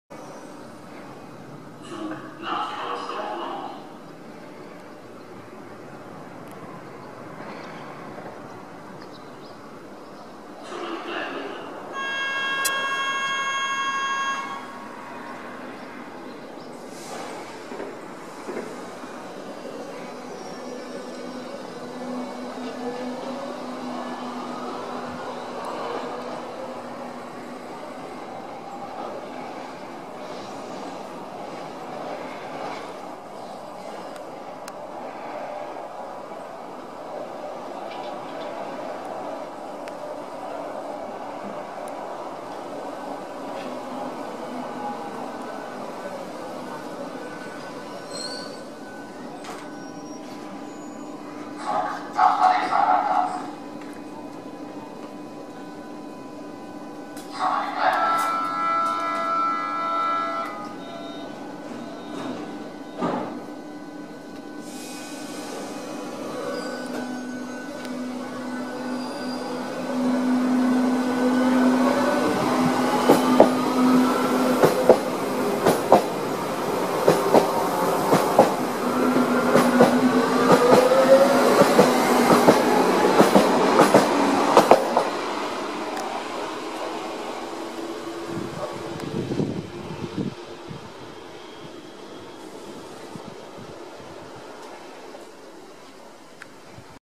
U5 Biesdorf-Süd (U-Bahn Berlin)
Baureihe H97 fährt zur Endstation Alexanderplatz, Type H97 goes to Alexanderplatz